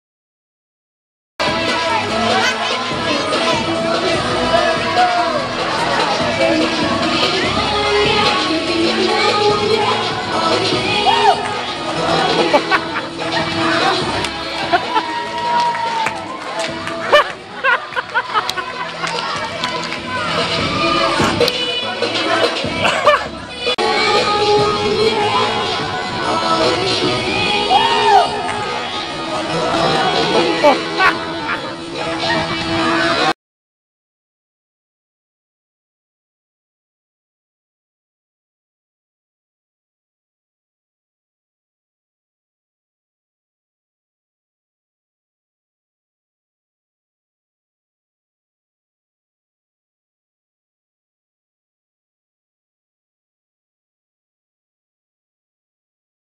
Roller Skate Fail - Lock in 2013
One more lasting memory of our great middle school lock-in with First Assembly of God in Cedar Rapids, IA...although this clip took place at a local roller skating rink.  I love middle school ministry!